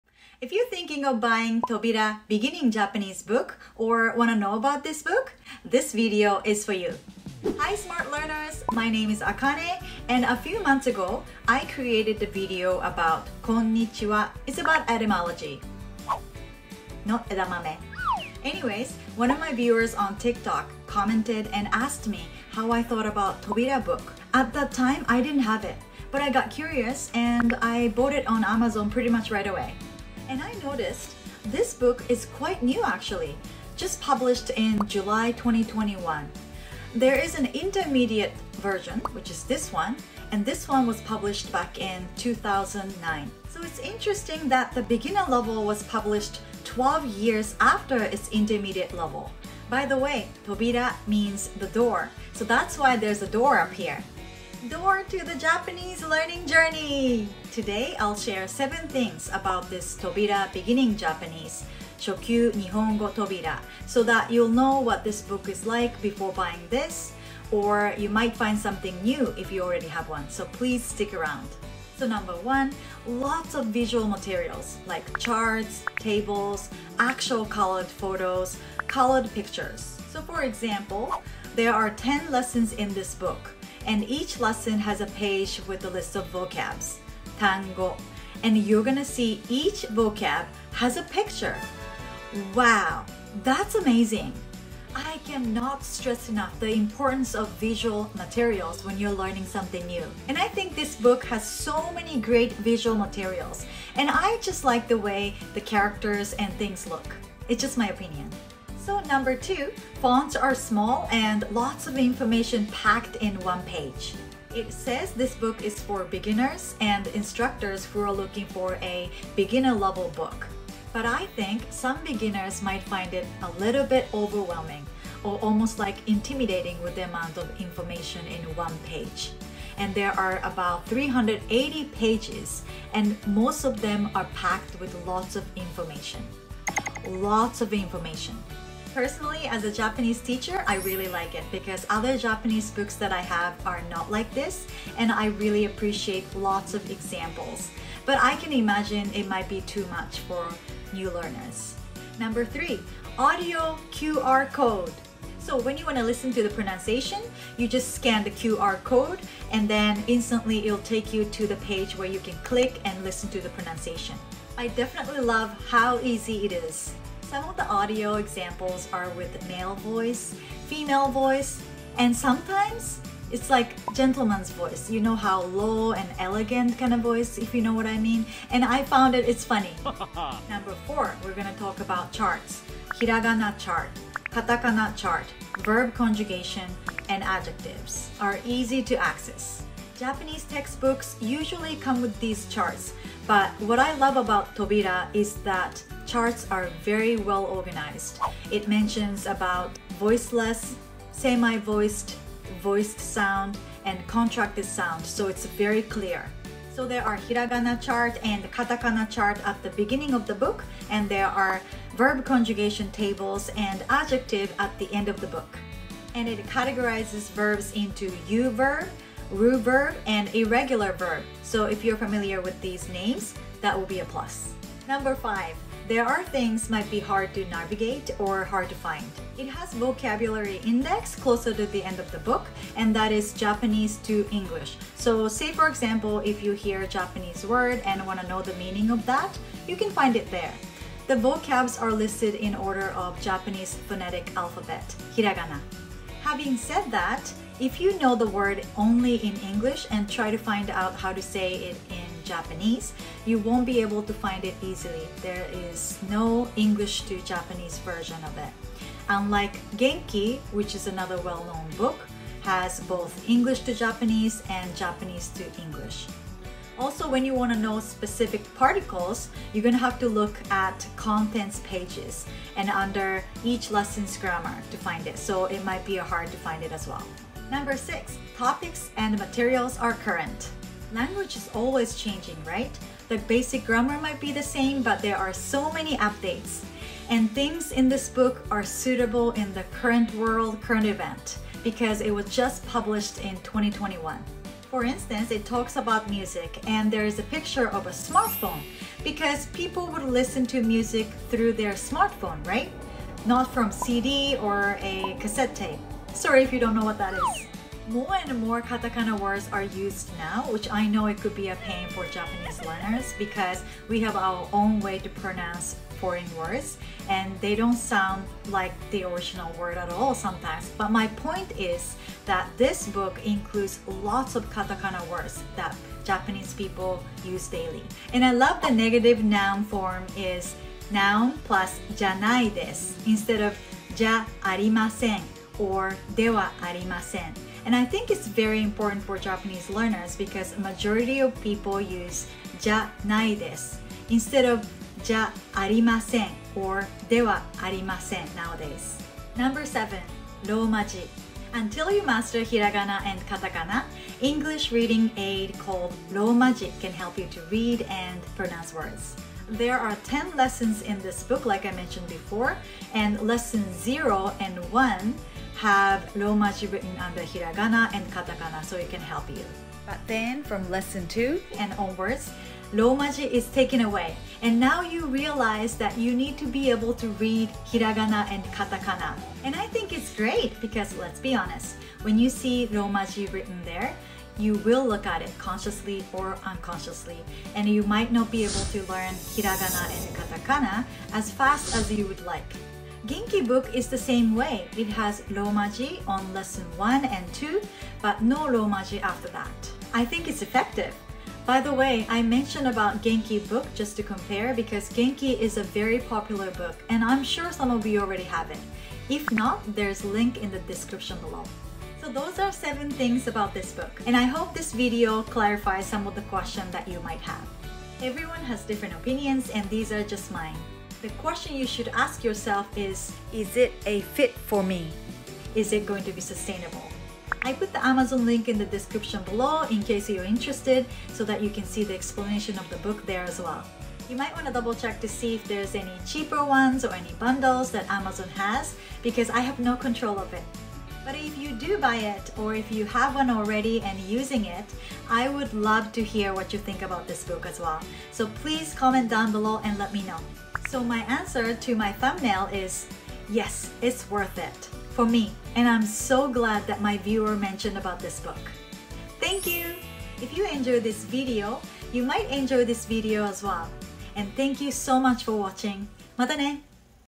7things to know about Tobira Beginning Japanese textbook
Japanese textbook review - Tobira Beginning Japanese 初級日本語とびら

If you are thinking of buying Tobira Beginning Japanese, or would like to know about this book, this video is for you.

TOBIRA BEGINNING JAPANESE 初級日本語とびら
Amazon USA

Amazon Japan

Amazon Canada

GENKI is a comparison book that I mentioned in this video.
(comparison book) GENKIげんき

Amazon USA

Amazon Canada

Amazon Japan

0:00 intro
1:12 #1. Lots of visual materials
1:49 #2. Lots of info with small fonts in a page
2:32 #3. Audio QR code
3:01 #4. East access to charts
3:45 #5. Things that are hard to navigate or to find
4:38 #6. Topics & materials are current
5:50 #7. Roma-ji ローマ字
7:30 Answer to my thumbnail question

Instagram @Natural Japanese with Acane